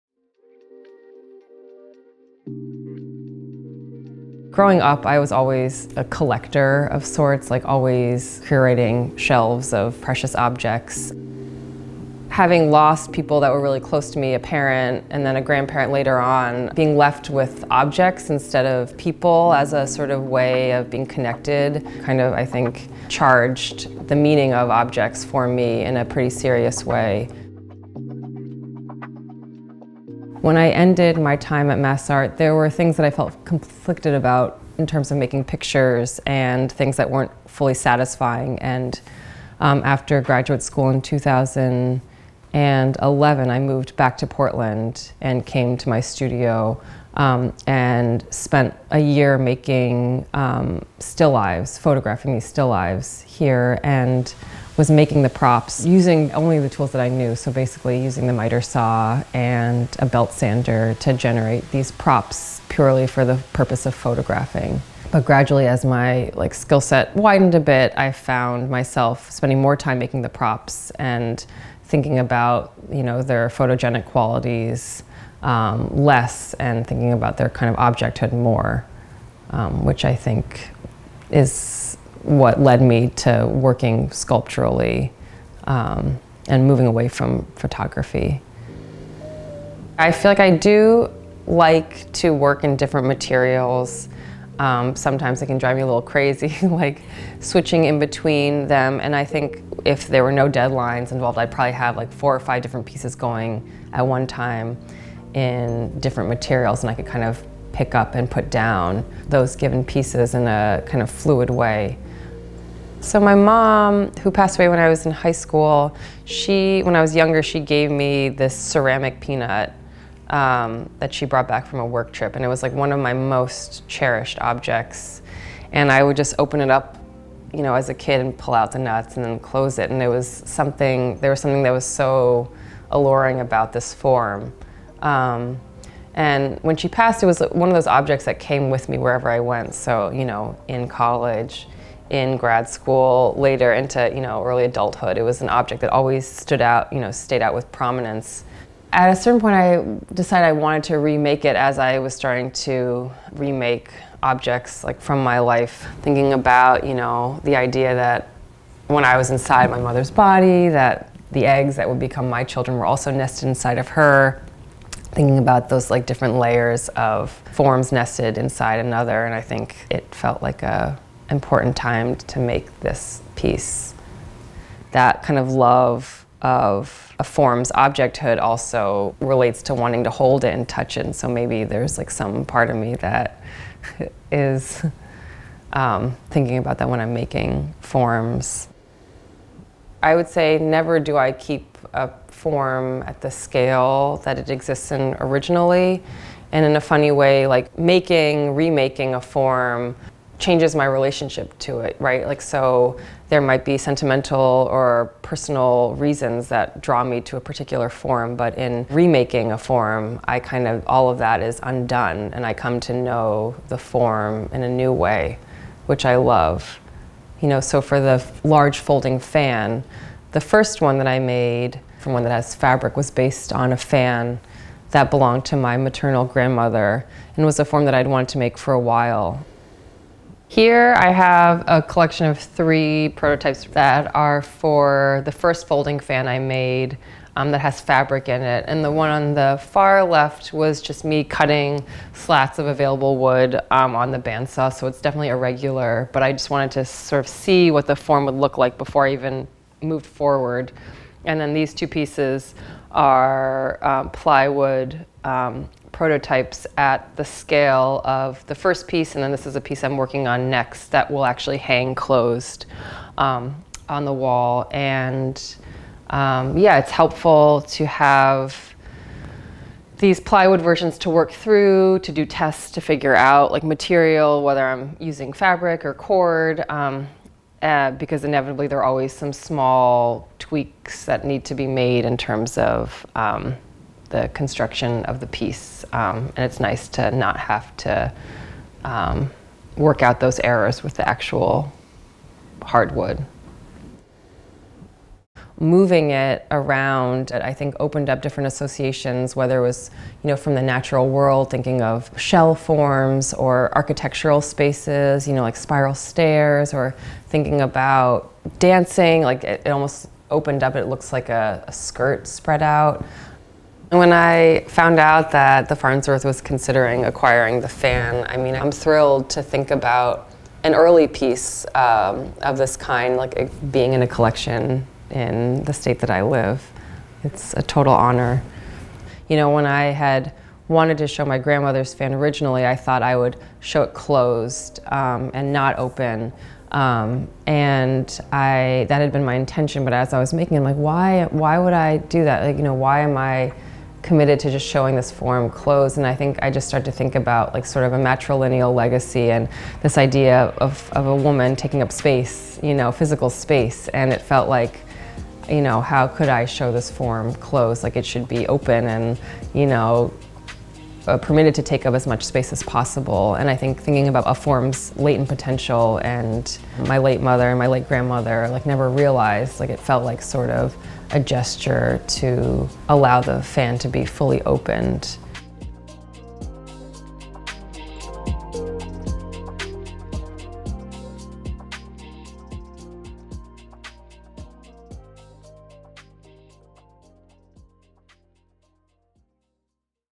In the Studio: Elizabeth Atterbury
The Farnsworth's In the Studio video series features in-depth interviews with artists whose work we’ve recently acquired or is in the Farnsworth Art Museum collection. These 5-6 minute videos—filmed in studio—offer insight into each artist’s process, as well as specific details surrounding their work in the Museum’s collection.